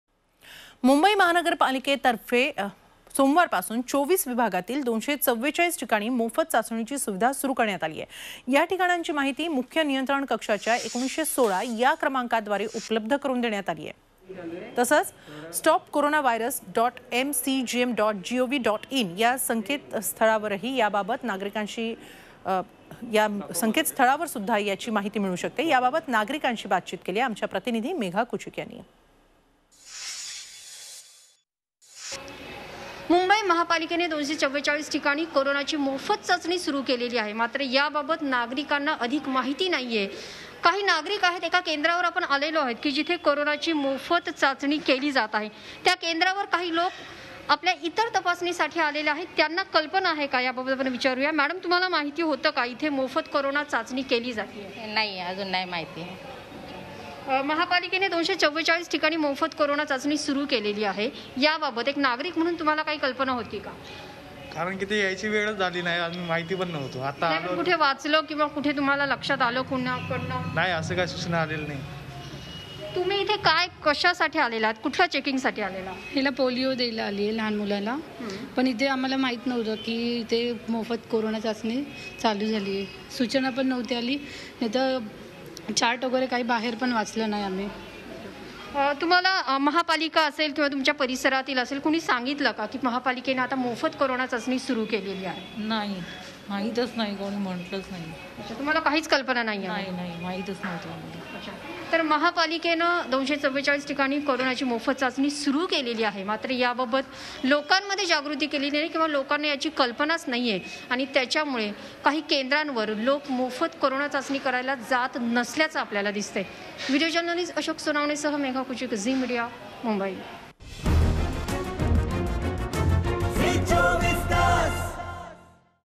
मुंबई | महापालिकेतर्फे २४४ ठिकाणी मोफत चाचणी
Mumbai | Mahapalika Organised Free Testing Center At 24 Wards